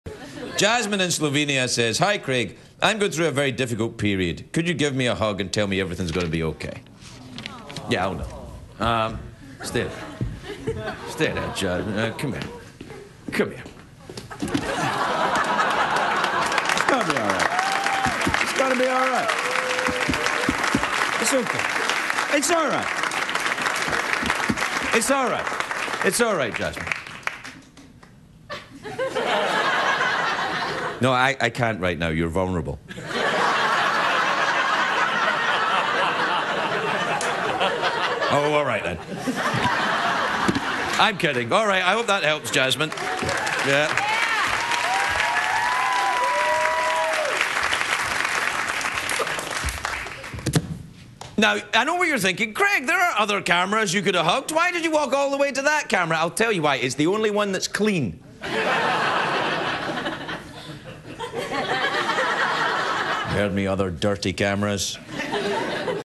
Craig Ferguson comforts viewer
Craig "hugs" a viewer from Slovenia who emails asking for a hug because she's having a difficult time. November 11, 2008